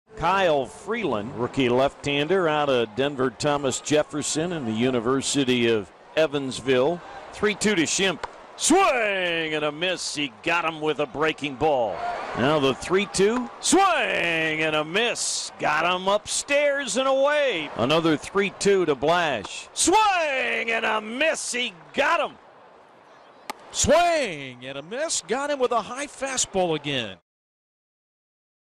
COL@SD: Freeland allows one run through six-plus
Kyle Freeland gives up one run on three hits while notching four strikeouts during his 6 1/3 inning outing